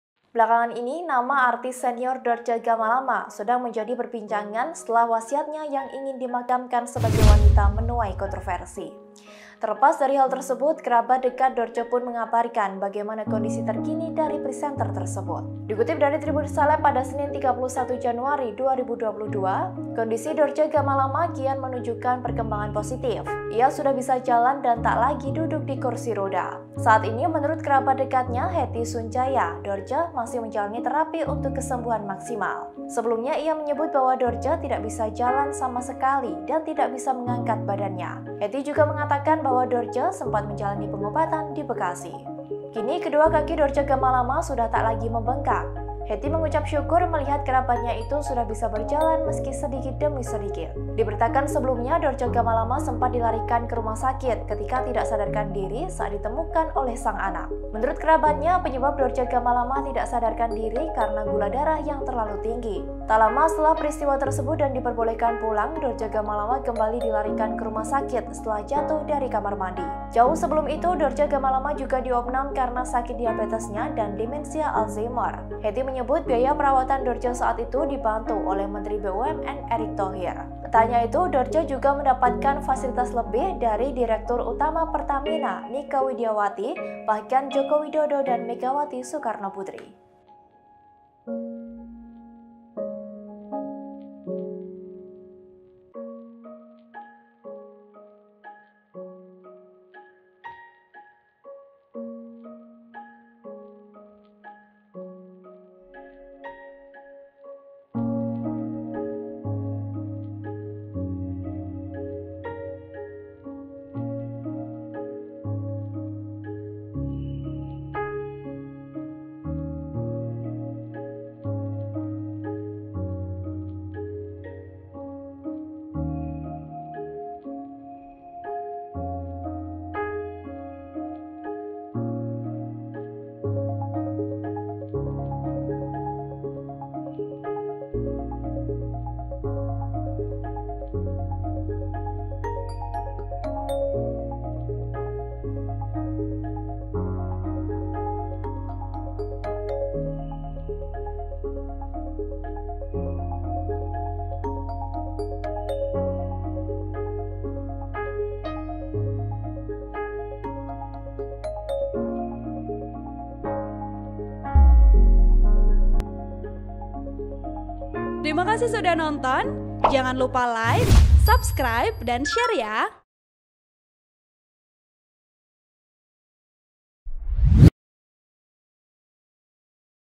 Jadi Perbincangan karena Ingin Dimakamkan sebagai Wanita, Begini Kondisi Terkini Dorce Gamalama
Belakangan nama artis senior Dorce Gamalama sedang menjadi perbincangan setelah wasiatnya yang ingin dimakamkan sebagai wanita menuai kontroversi.

Terlepas dari hal tersebut, kerabat dekat Dorce pun mengabarkan bagaimana kondisi terkini dari presenter tersebut.

[Yas]